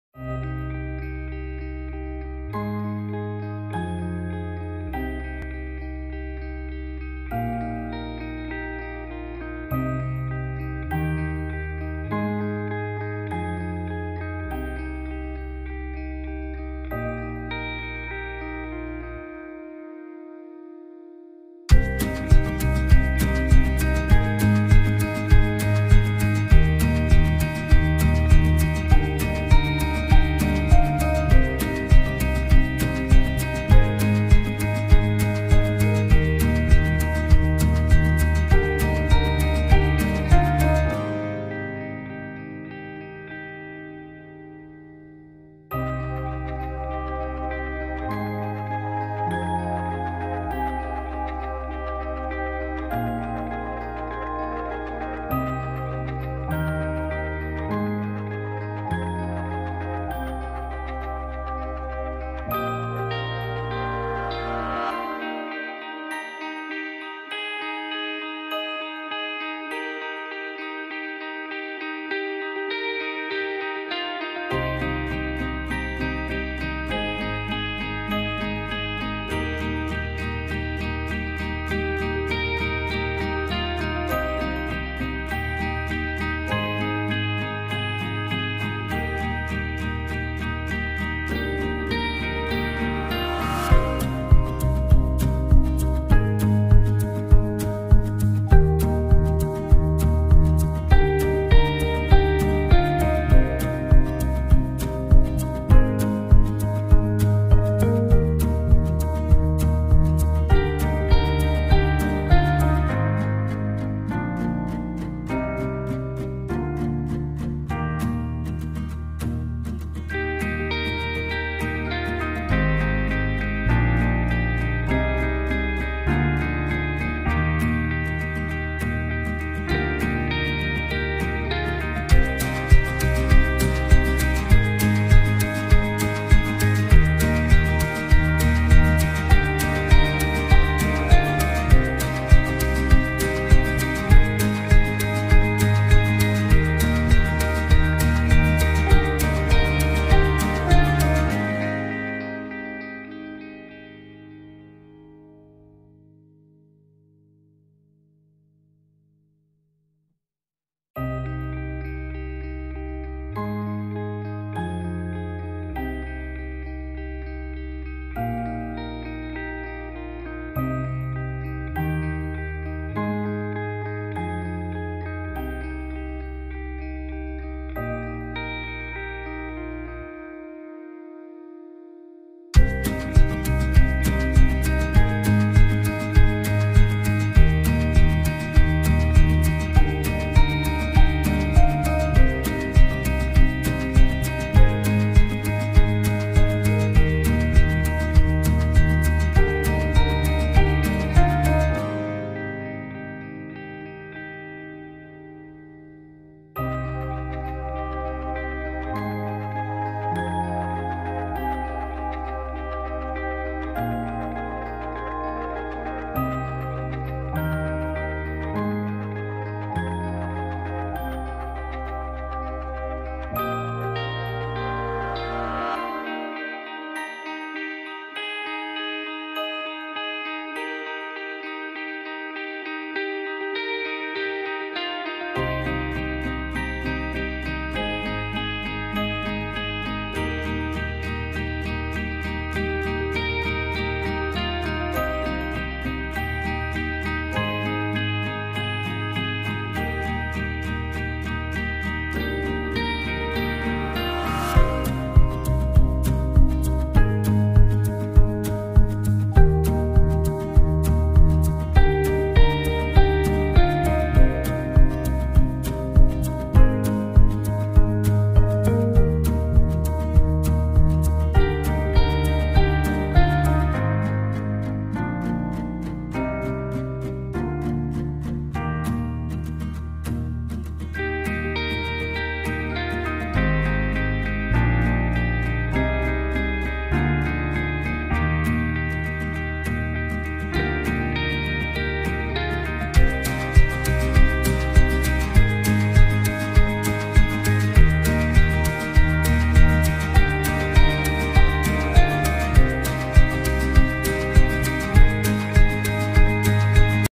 supla yapılışı kolay ve kibar yeni model makrome Amerikan servisi super knitting crochet placemat
istediğiniz büyüklükte yapabilirsiniz